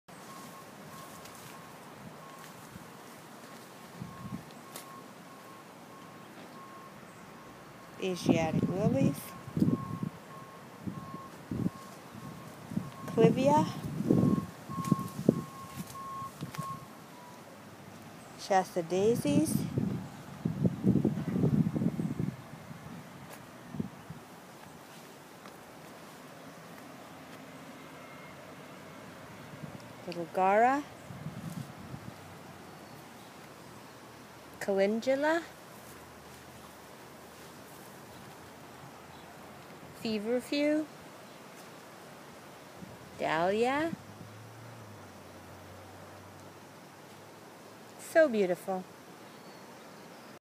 In the garden with Cindy McGonagle
Summer in full bloom in a garden in Yachats Oregon